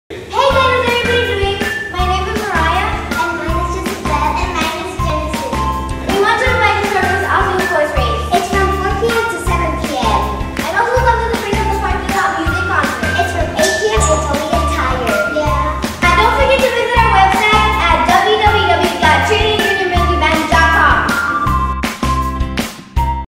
Invitation to Praise On The Parking Lot Christian music concert!
TJMB invites to our 1st music concert this July 27th!
Happening at our church parking lot! Trinity Baptist Church 505 Old Alice Rd Brownsville, Tx. 78520